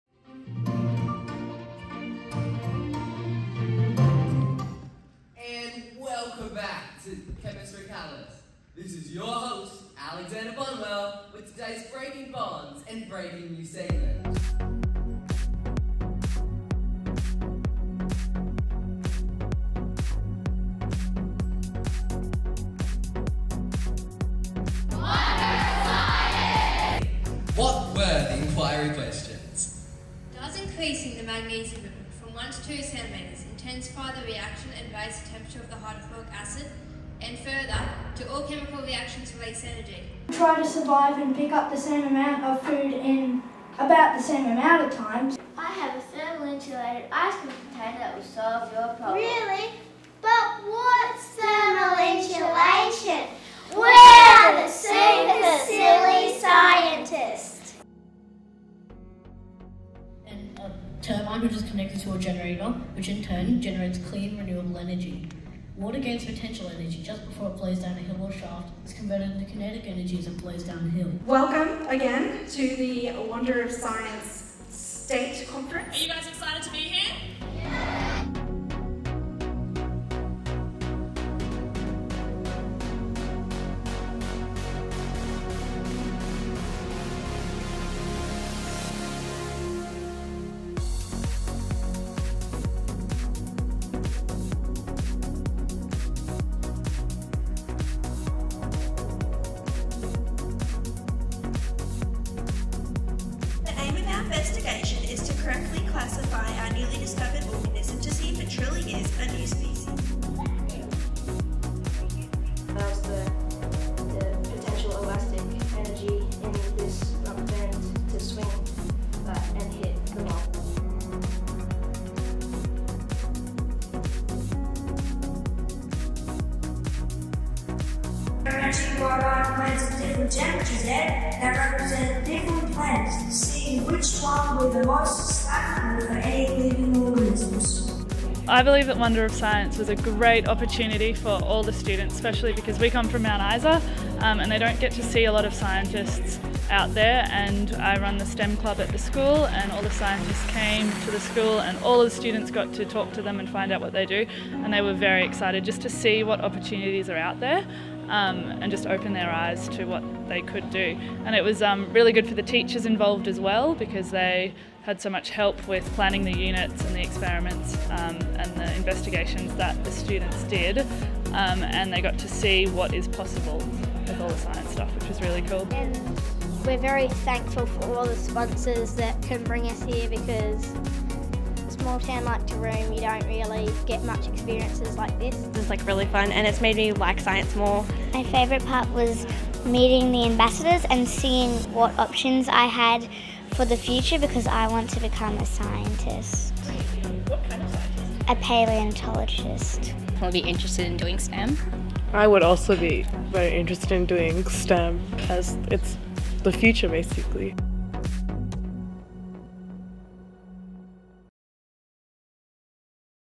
Wonder of Science
Wonder of Science fosters a STEM culture that empowers and increases capability for positive student and societal outcomes via access to high-quality, evidence-based STEM education. Specifically, a culture where all students, their teachers, parents and other community members understand and value the importance and potential of Science, and STEM pathways and careers.

STEM Inquiry - Challenging, real-world tasks aligned to the Australian Curriculum

Students develop critical and creative thinking skills as they are challenged to develop a group response to a real-world problem or scenario. Representative teams are chosen to attend regional student conferences to present their response and showcase their work.

Young Science Ambassadors - Bringing STEM to life
Selected PhD researchers from leading Queensland universities visit participating schools to work with students on their challenge tasks, and support them to develop a group presentation. The ambassadors are inspiring role models for both students and teachers. The ambassadors support implementation of Wonder of Science, and judge presentations at the student conferences.

Student Conferences - Aspirational regional and state student showcases
Representative teams from participating schools are invited to present their challenge task response and compete against other schools at a one-day regional student conference. In addition, students have the opportunity to participate in ‘Speed-meet-the-Scientist’ activities and workshops.

Strategic Partnerships - Linking schools, universities, industry groups, non-profit organisations & government agencies

Wonder of Science fosters partnerships between schools and the wider community through our partner organisations. Our university partners host student conferences and provide students for the Young Science Ambassador program. Industry partners attend student conferences to provide workshops and judge student presentations.

Acknowledgements: Thank you to Arrow Energy for providing footage of 2023 State Conference included in this video.